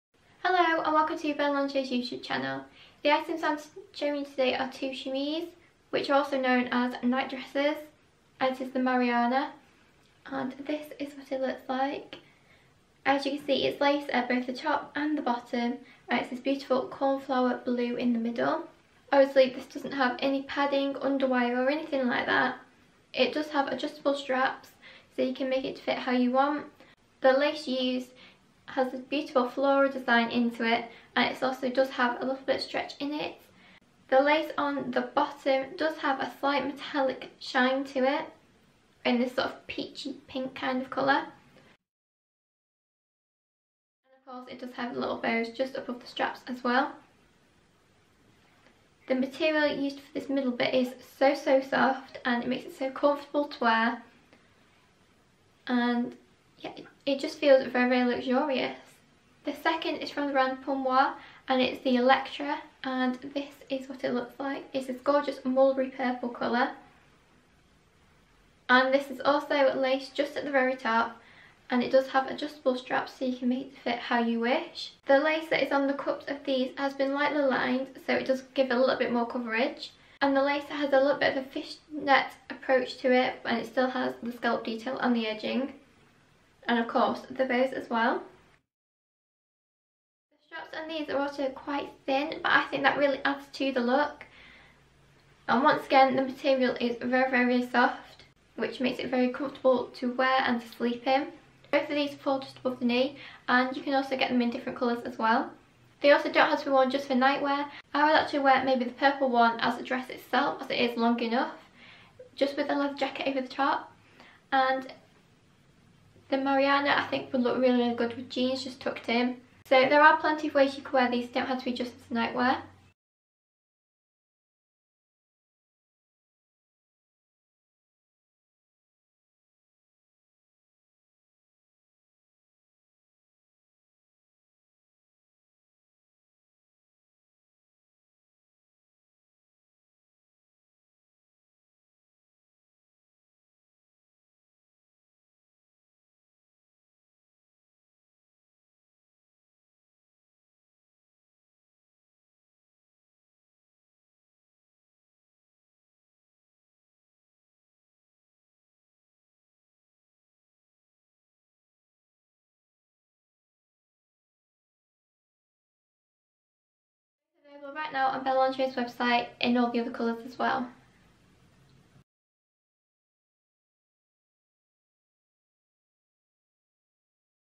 Fantasie Marianna Chemise & Pour Moi Electra Chemise | Belle Lingerie Review
Chemises make excellent nightwear and today we have two to show you! In this review, we discuss the key features and benefits of the Fantasie Marianna Chemise and also the Pour Moi Electra Chemise.

Did you know, you can also style your chemise with a pair of jeans and your favourite jacket to create a new look for your wardrobe?